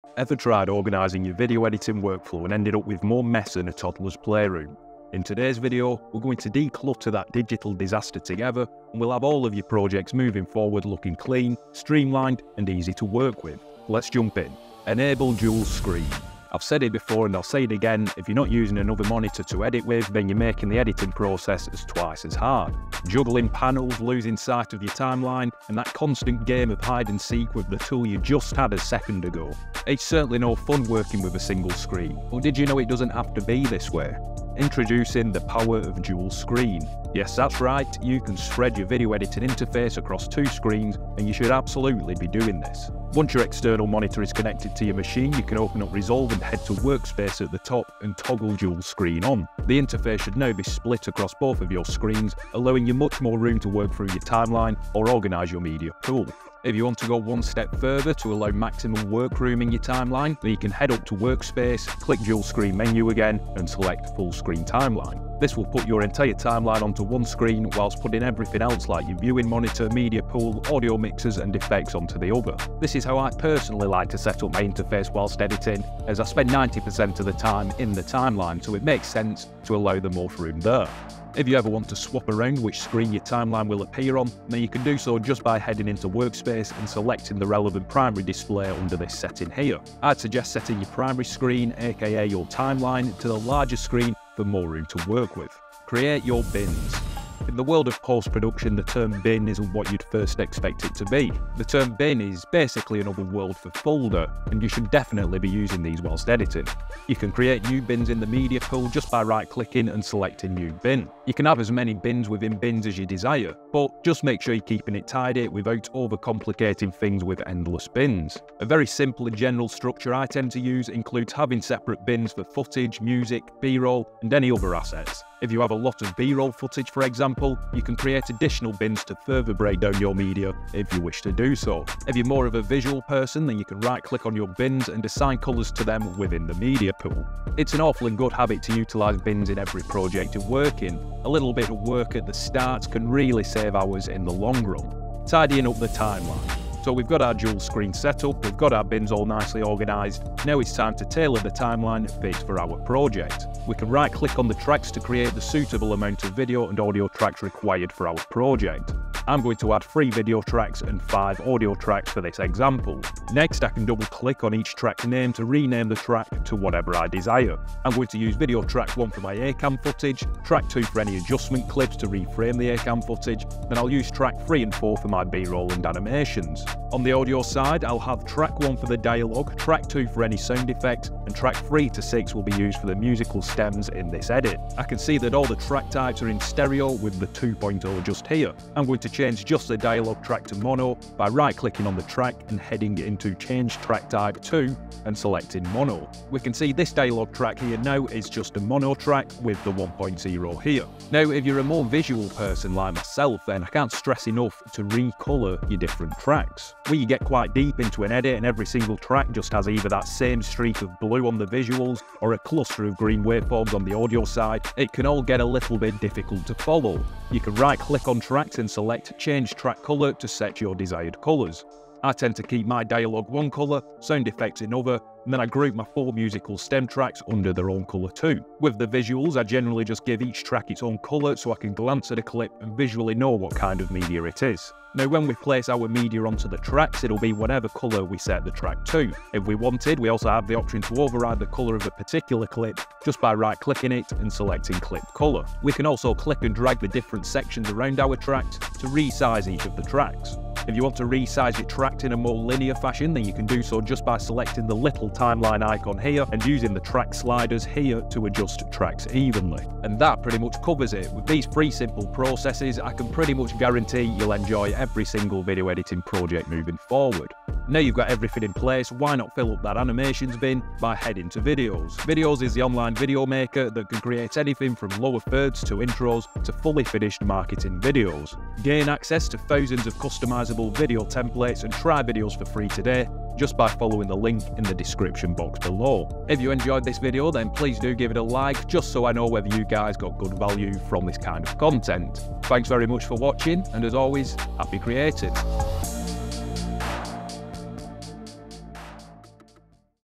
Efficient Video Editing Workflow (DaVinci Resolve 2024)
Tired of navigating through a chaotic video editing workflow? Join Tom Ainscough as he guides you through a step-by-step process to streamline your video editing setup using DaVinci Resolve. In this tutorial, we'll cover the essentials of setting up a dual-screen workspace, organizing your media using bins, tidying up the timeline, and customizing track colors for easy navigation. Whether you're a beginner or a seasoned editor, these tips will revolutionize your workflow, making your video editing process cleaner, more streamlined, and efficient.

🕒 Timestamps:

[0:00] Introduction to Streamlining Video Editing
[0:16] Setting Up Dual Screen in DaVinci Resolve
[1:36] Utilizing Bins for Media Organization
[2:25] Tailoring and Organizing the Timeline
[4:20] Final Tips for an Efficient Workflow

🧠 What You'll Learn:

Learn how to effectively use dual screens for video editing.
Discover the best practices for using bins to keep your media organized.
Tips on customizing your timeline for a smoother editing experience.
Practical advice for both beginners and experienced editors.